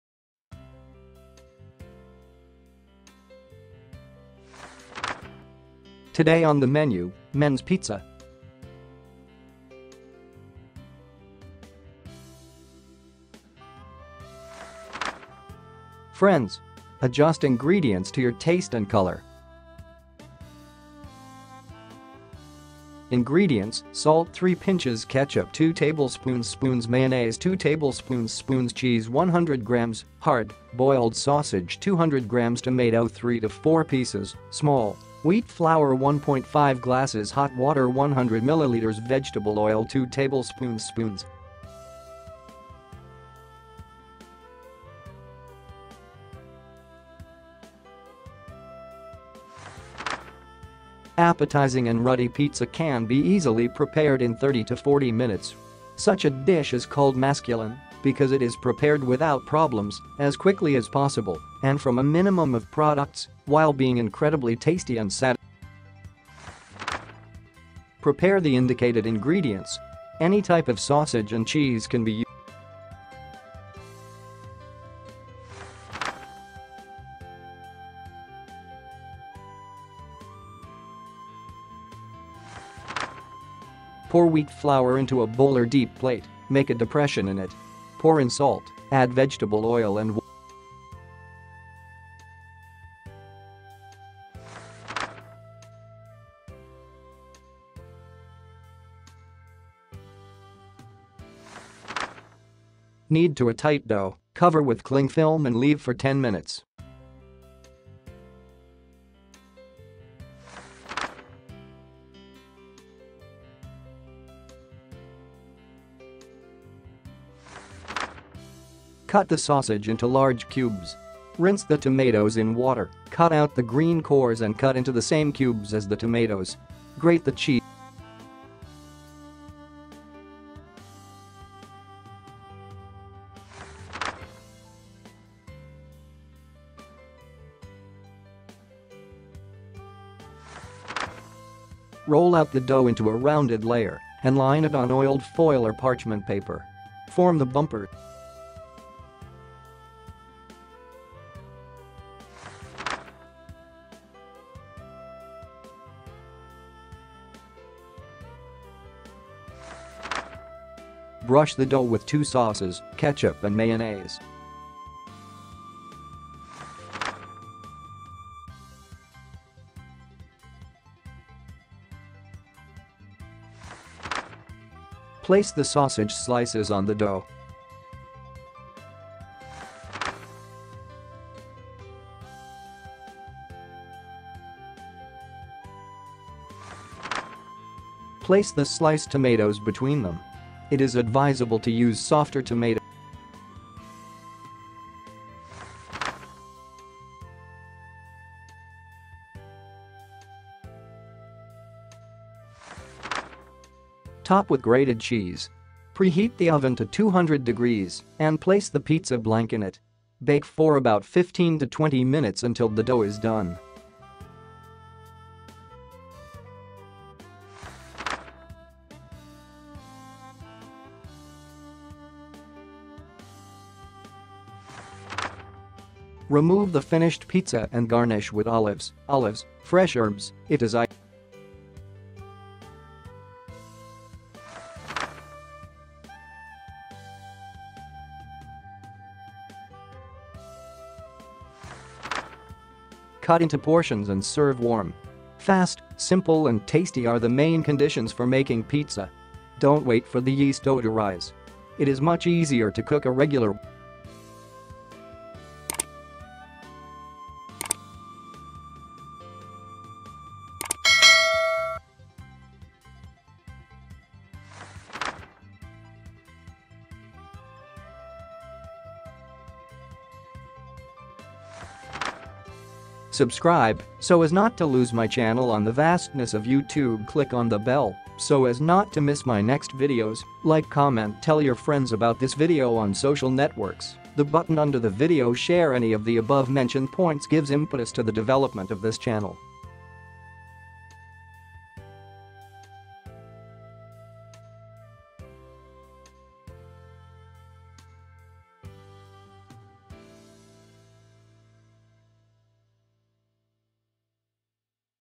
"Men's" Pizza / Book of recipes / Bon Appetit
✅ "Interesting" salad 👇

Appetizing and ruddy pizza can be easily prepared in 30-40 minutes. Such a dish is called masculine because it is prepared without problems, as quickly as possible and from a minimum of products, while being incredibly tasty and satisfying.

Ingredients:
* Salt - 3 Pinches
* Ketchup - 2 tbsp. spoons
* Mayonnaise - 2 Tbsp. spoons
* Cheese - 100 grams (hard)
* Boiled sausage - 200 Grams
* Tomato - 3-4 Pieces (small)
* Wheat flour - 1.5 glasses
* Hot water - 100 Milliliters
* Vegetable oil - 2 tbsp. spoons

👉 Step by step cooking 👇

 1. Prepare the indicated ingredients. Any type of sausage and cheese can be used.

2. Pour wheat flour into a bowl or deep plate, make a depression in it. Pour in salt, add vegetable oil and water.

3. Knead to a tight dough, cover with cling film and leave for 10 minutes.

4. Cut the sausage into large cubes. Rinse the tomatoes in water, cut out the green cores and cut into the same cubes as the tomatoes. Grate the cheese on a fine mesh grater.

5. Roll out the dough into a rounded layer and line it on oiled foil or parchment paper. Form the bumpers.

6. Brush the dough with two sauces, ketchup and mayonnaise.

7. Place the sausage slices on the dough.

8. Place the sliced ​​tomatoes between them. It is advisable to use softer tomatoes.

9. Top with grated cheese. Preheat the oven to 200 degrees and place the pizza blank in it. Bake for about 15-20 minutes until the dough is done.

10. Remove the finished pizza and garnish with olives, olives, fresh herbs - if desired.

11. Cut into portions and serve warm. Fast, simple and tasty are the main conditions for making pizza. Don't wait for the yeast dough to rise. It is much easier to cook a regular one and bake all the cuts on it.